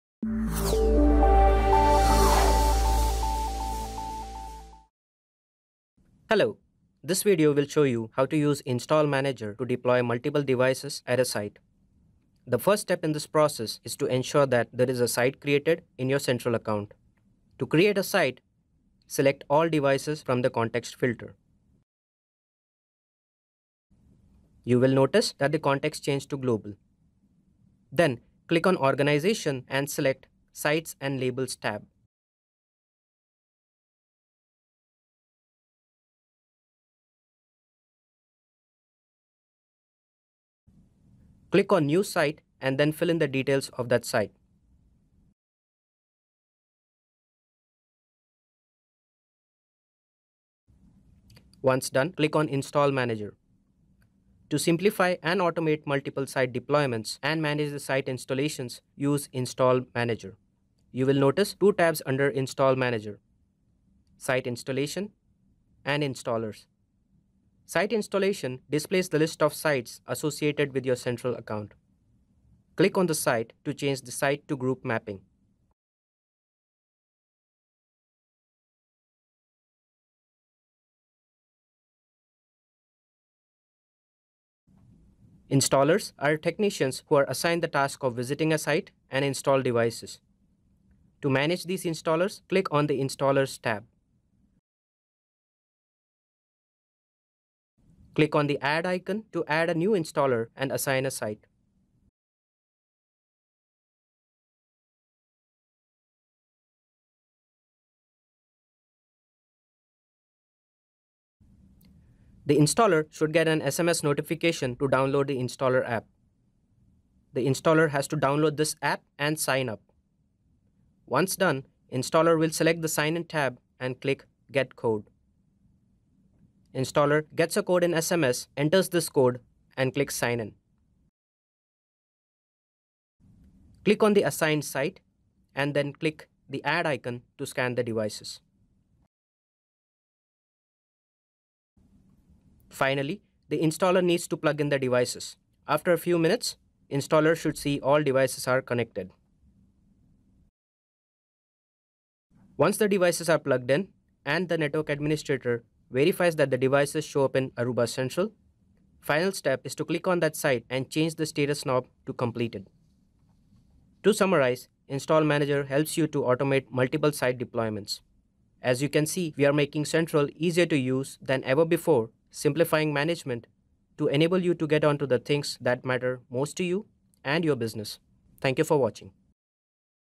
Aruba Central Enhancements (SD-Branch) - Device Provisioning Using Install Manager (Part-2)
This video shows device provisioning using Install Manager, which automates site deployment in Aruba Central.